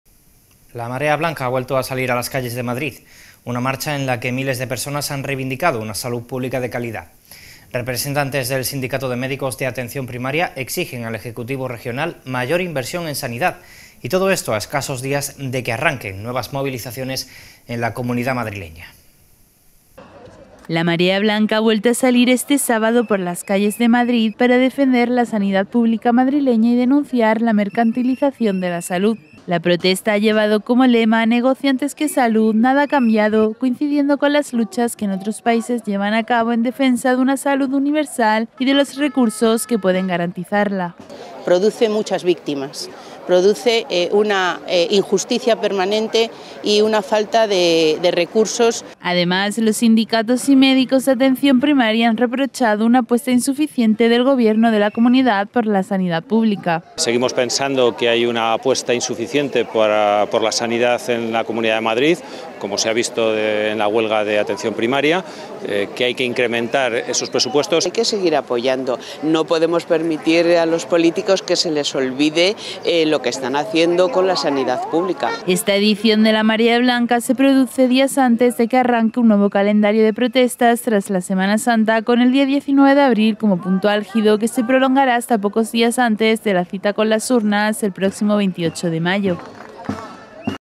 🔴Noticia - La 'Marea Blanca' vuelve a las calles de Madrid
La Marea Blanca ha vuelto a salir este sábado por las calles de Madrid para defender la Sanidad Pública madrileña y denunciar la "mercantilización" de la salud.

English:

The White Tide has come out again this Saturday through the streets of Madrid to defend Madrid's Public Health and denounce the "commodification" of health.

Live streaming